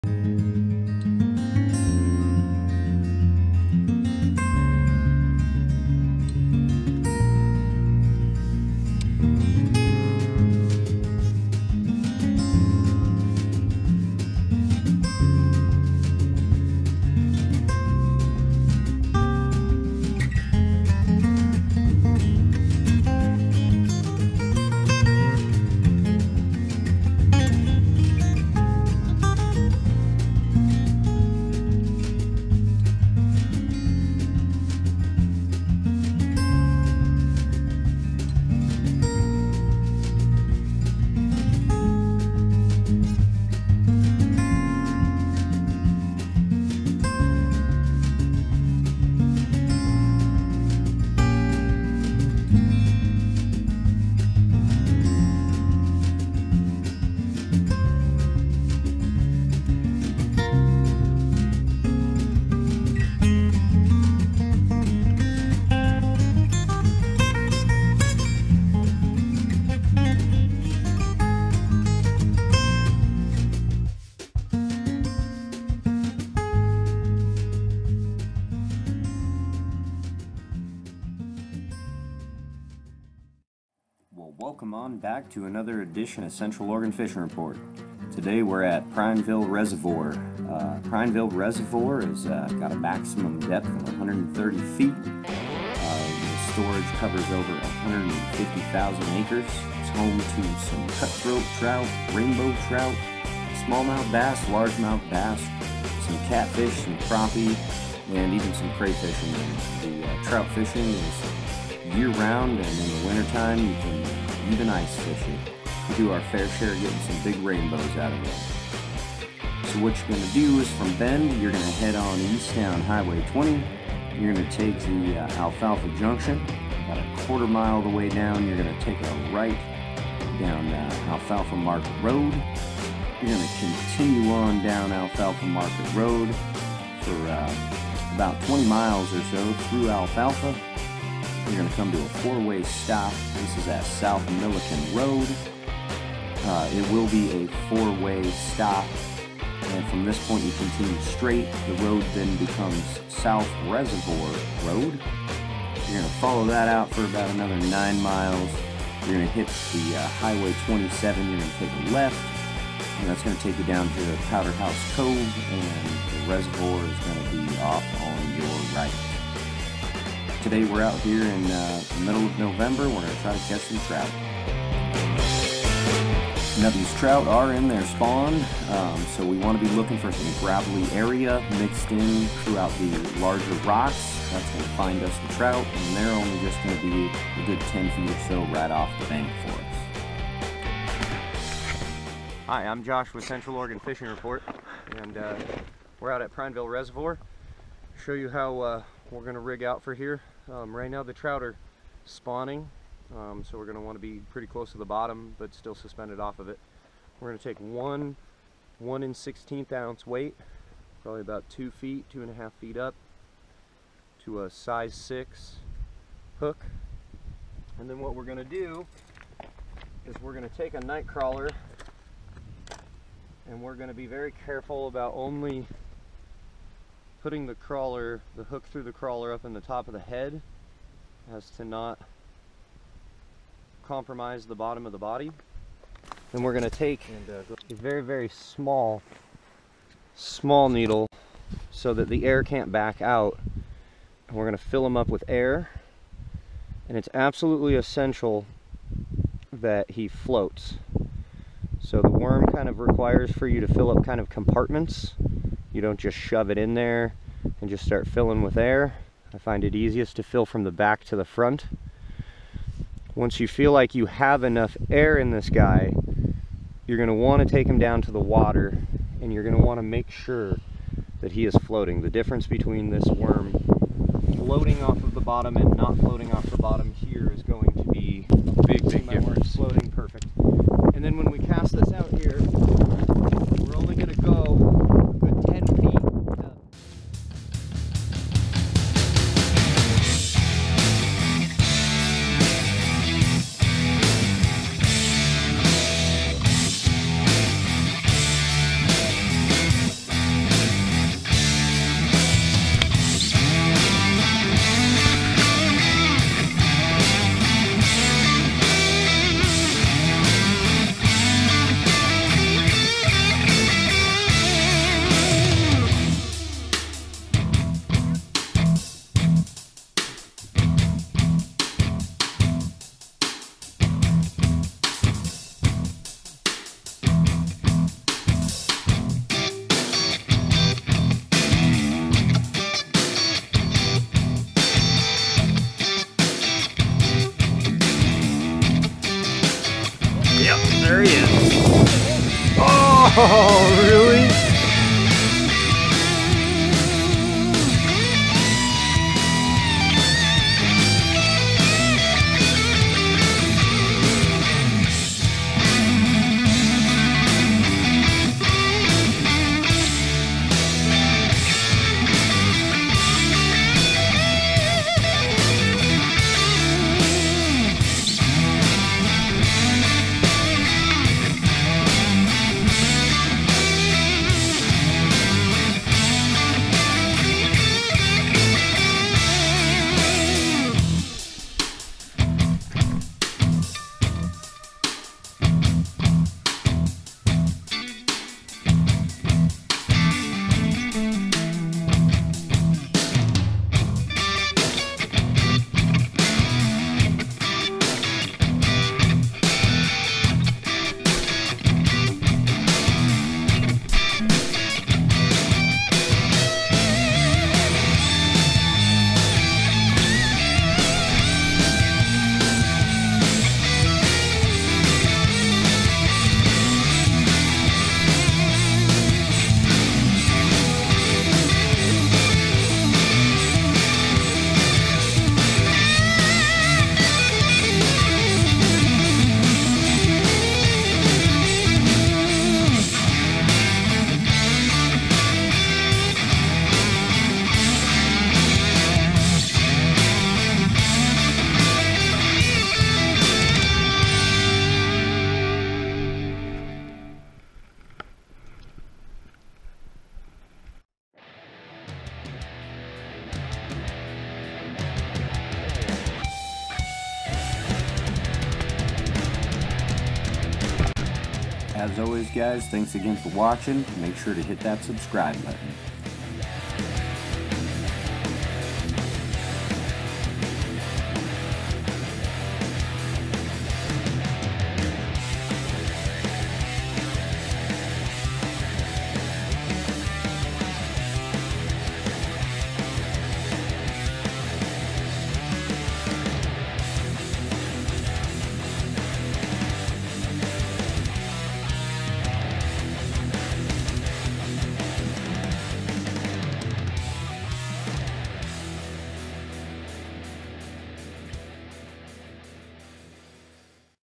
How To Catch - Winter Trout - Prineville Reservoir
The Central Oregon Fishing Report Team Shows You How To Catch Large Winter Rainbow Trout At Prineville Reservoir From A Bank Aspect.